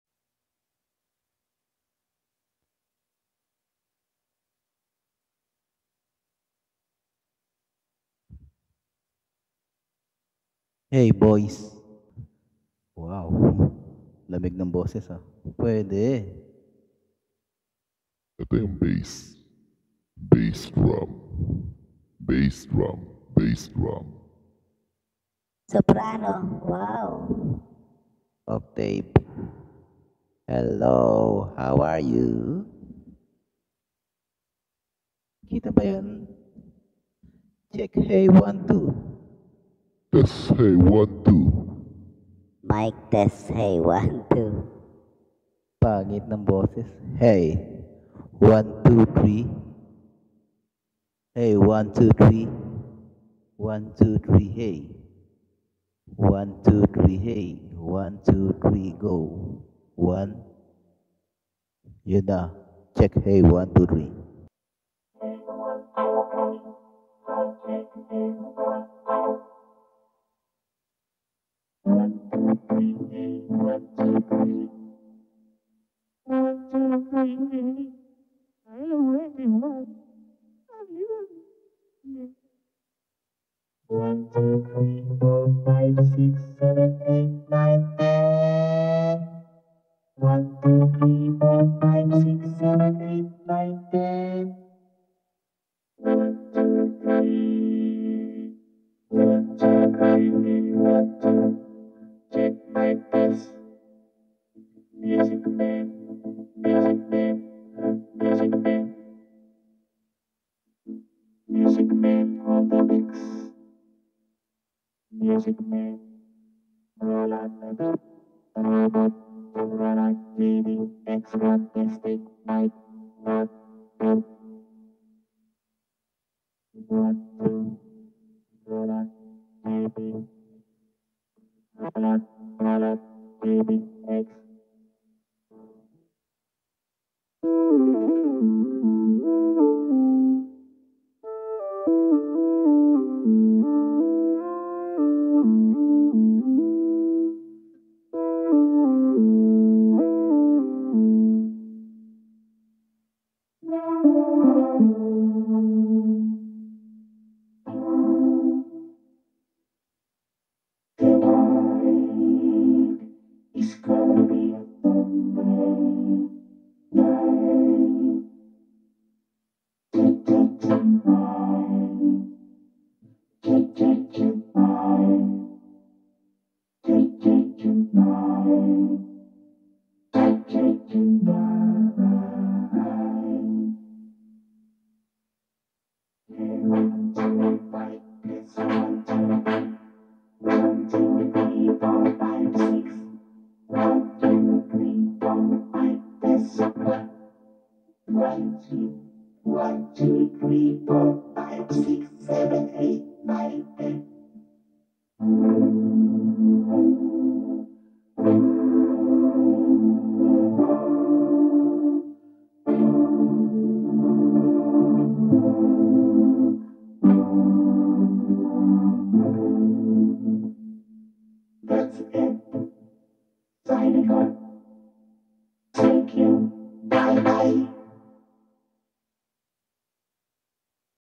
Roland JD-Xi using Vocoder (Part1)
Here is my 1st  try using vocoder in this gear
I Hereby declare that I do not have the right to this music.

the ff video and music in this content are for entertainment only

(Muzikman channel)